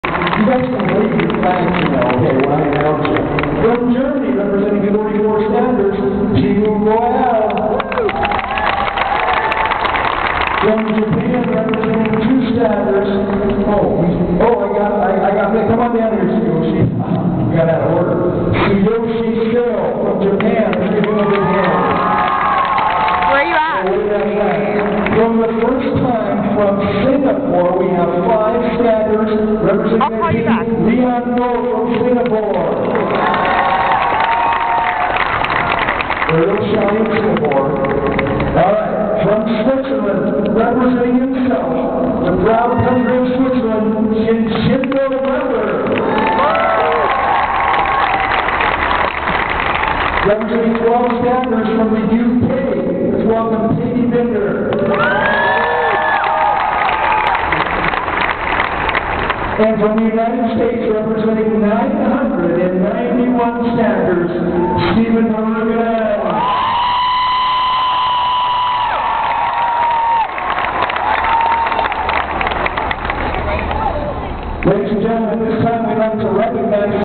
2008 WSSA World Sport Stacking Championships Footage 1
This video is from the opening ceremonies on Saturday, April 5th. Presentation of flags for each country.

Note: I did start this video a few seconds late. Sorry. But the first flags were Canada and Columbia.

Footage from the 2008 WSSA World Sport Stacking Championships. There are more so check them all out!